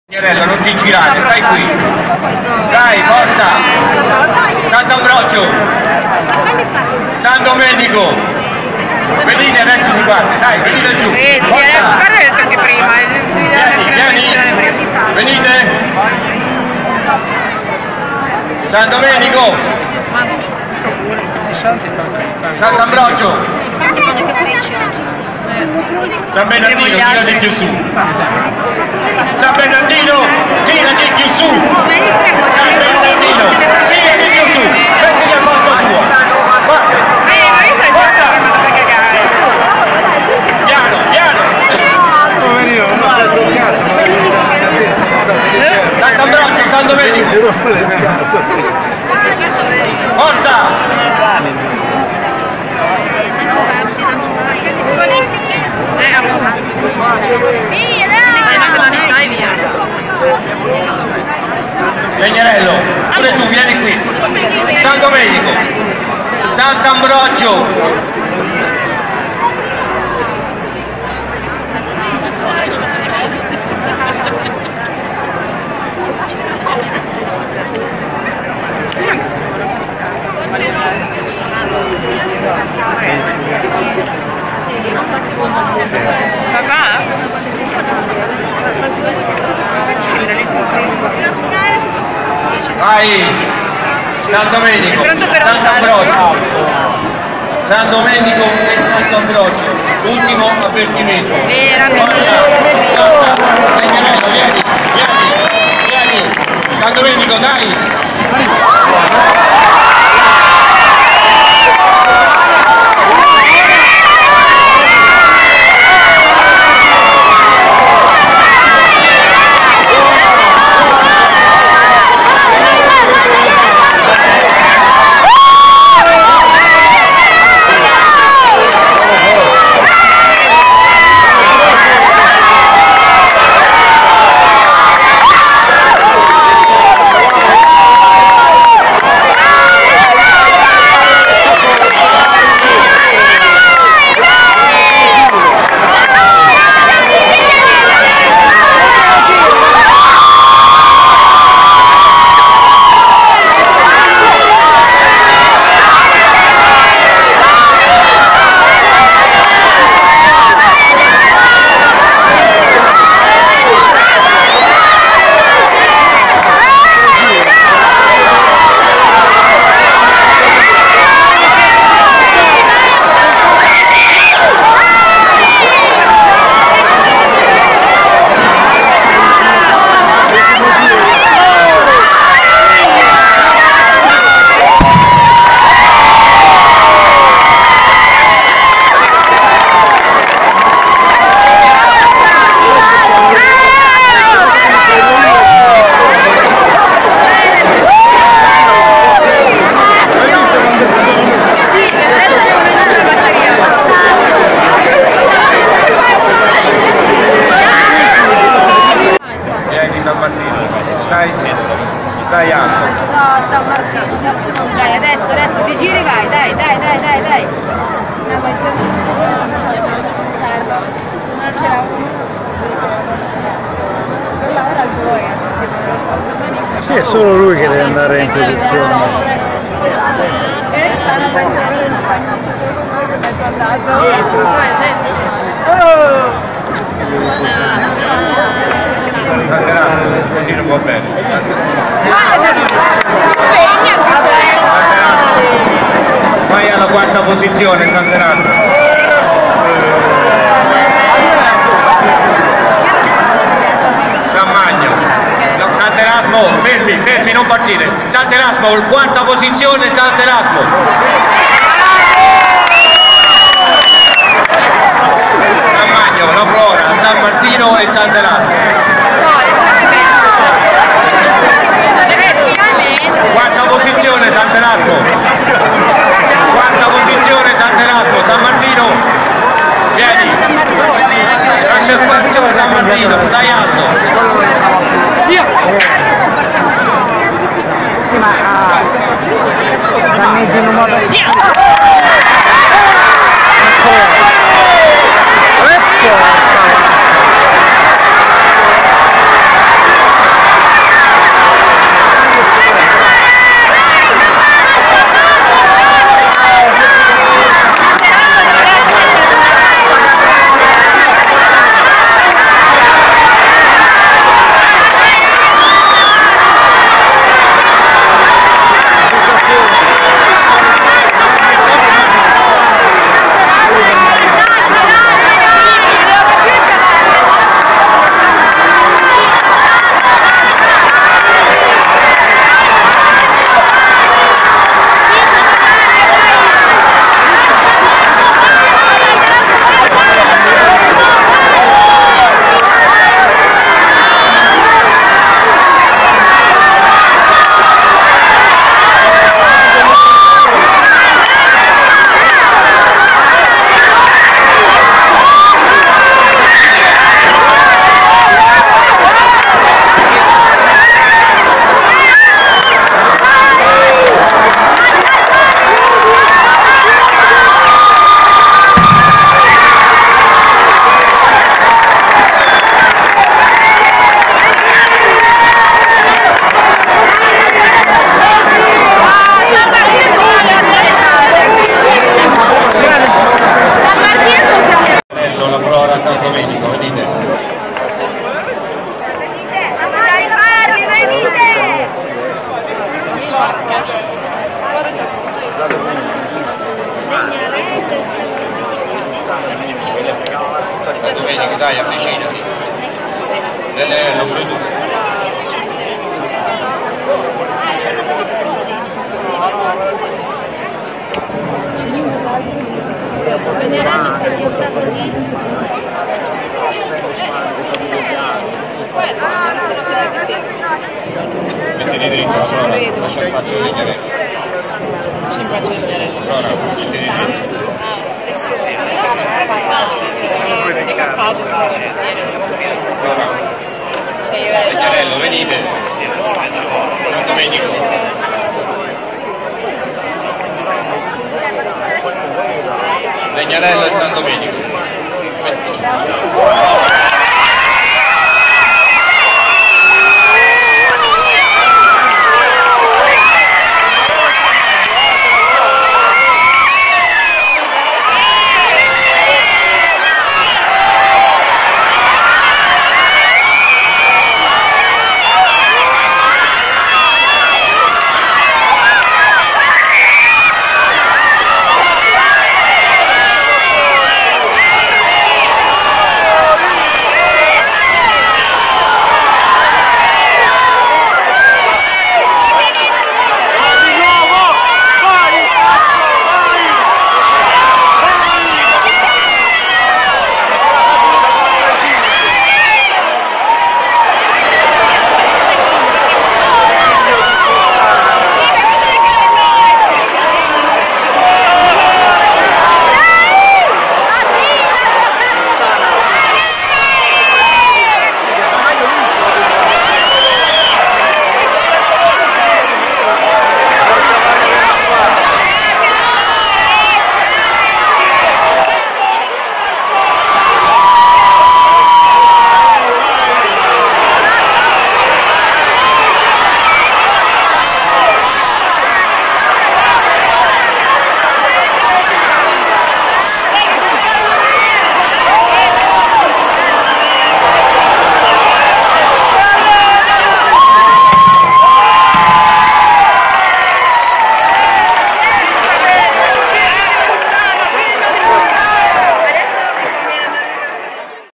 Palio di Legnano 2019 semifinali e finalissima
paliodilegnano legnano, estenuante giornata sotto un sole pazzesco
La giornata di sfide